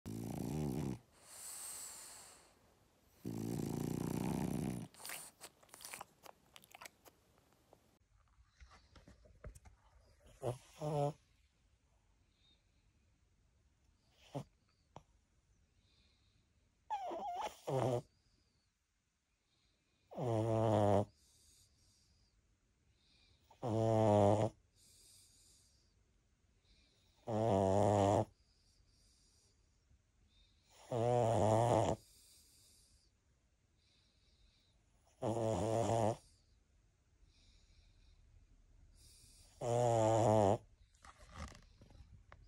Frenchies Snoring With Their Cuddle Buddy || ViralHog
Occurred on December 10, 2022 / Aberdeen

"Me and my dogs sleeping"